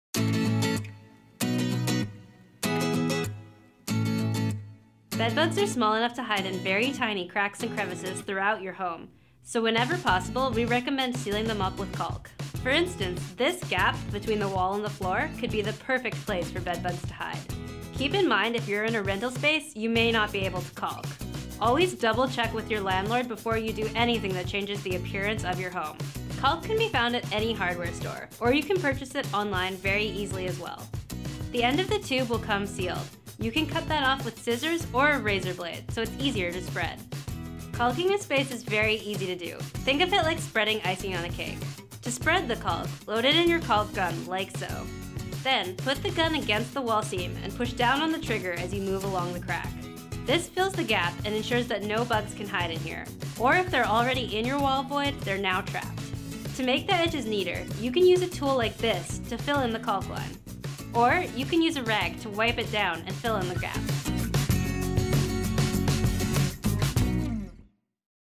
STEP #5: SEALING CRACKS AND CREVICES | Get rid of Bed Bugs
🕷️Since bed bugs are very small it’s easy for them to hide and nest in just about any little crack or crevice. They can also use cracks as a route to travel through your home or from apartment to apartment in multi-unit dwellings.🕷️

✅ Caulking Applicator

✅ Caulk Tool

🕷️🎓LEARN HOW TO GET RID OF BED BUGS🎓🕷️
Have Bed Bugs? Check out our FREE, effective 8-Step Approach to get rid of bed bugs from your home.